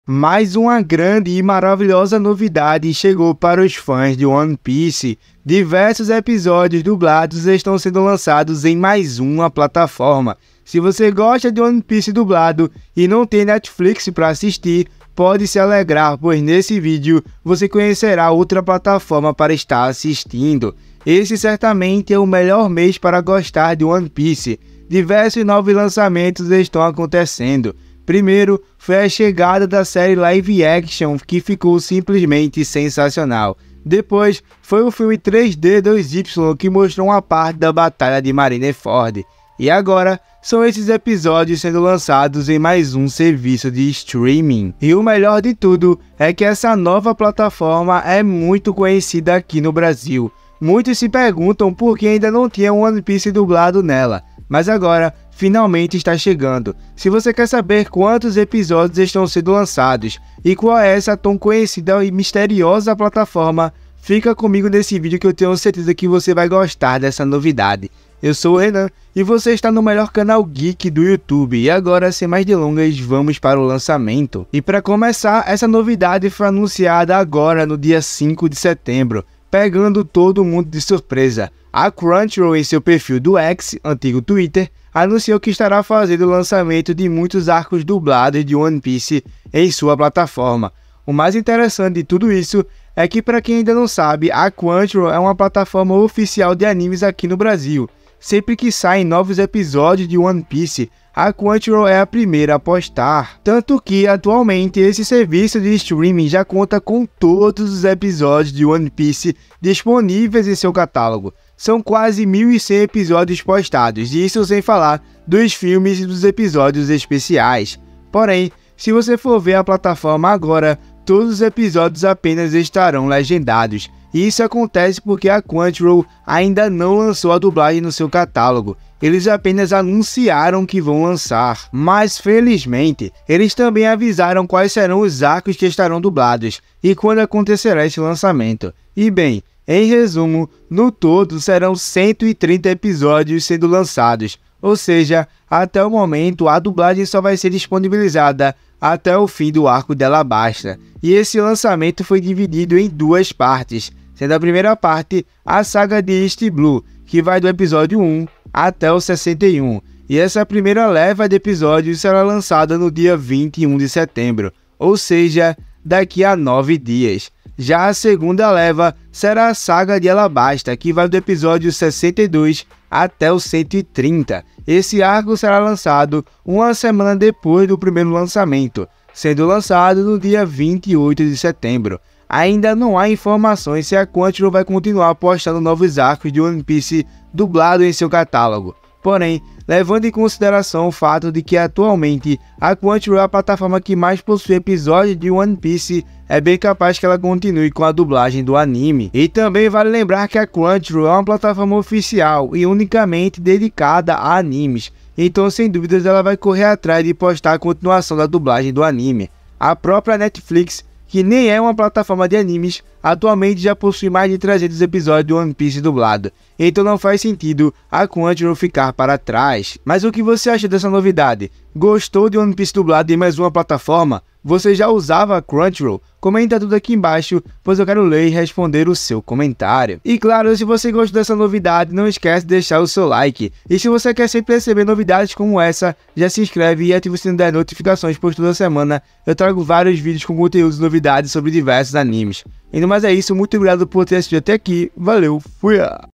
BOMBA! NOVOS EPISÓDIOS DE ONE PIECE DUBLADO FORAM LANÇADOS EM OUTRA PLATAFORMA! E AGORA NETFLIX?
Mais uma grande e maravilhosa novidade chegou para os fãs de One Piece! Diversos episódios dublados estão sendo lançados em mais uma plataforma. Se você gosta de One Piece dublado e não tem Netflix para assistir, pode se alegrar pois nesse vídeo você conhecerá outra forma de estar assistindo!

Esse certamente é o melhor mês para gostar de One Piece. Diversos novos lançamentos estão acontecendo. Primeiro foi a chegada da série live action que ficou simplesmente sensacional, depois foi o filme 3d2y que mostrou uma parte da batalha de marineford, e agora, são esses episódios sendo lançados em mais um serviço de streaming.

E o melhor de tudo, é que essa nova plataforma é muito conhecida aqui no Brasil, muitos se perguntavam por que ainda não tinha One Piece dublado nela, mas agora, finalmente está chegando!